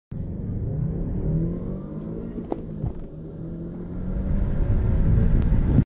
1999 Subaru Legacy GT-B Twin Turbo wagon 0-60 0-100
Bone stock, poor launch